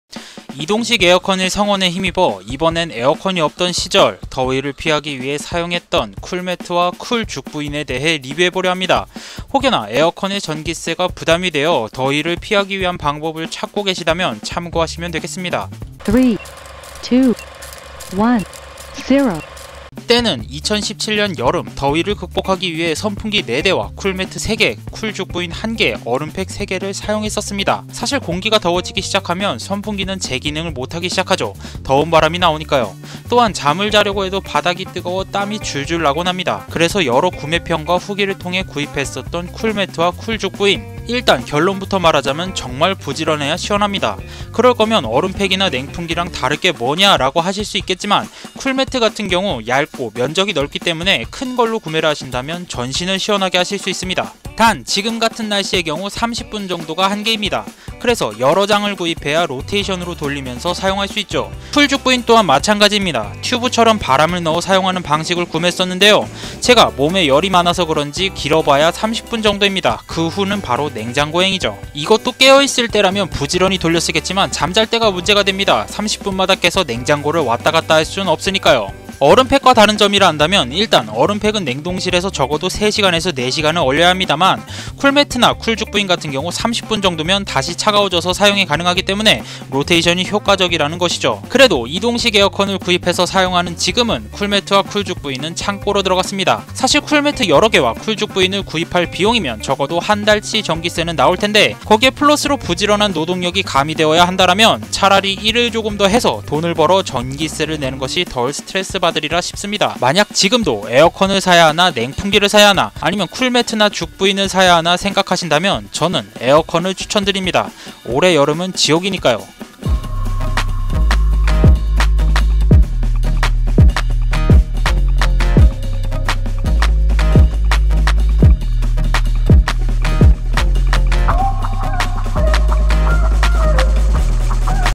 부지런하다면 추천하는 여름 용품 쿨매트와 쿨죽부인
쿨매트,쿨죽부인 - 100초로 모든 것을 리뷰한다 100초 엉터리뷰!

이번엔 이동식 에어컨의 뒤를 이어 더 저렴하게 여름을 보낼 수 있는 용품들을 한 번 준비해 보았습니다. 이름하야 쿨매트와 쿨죽부인!!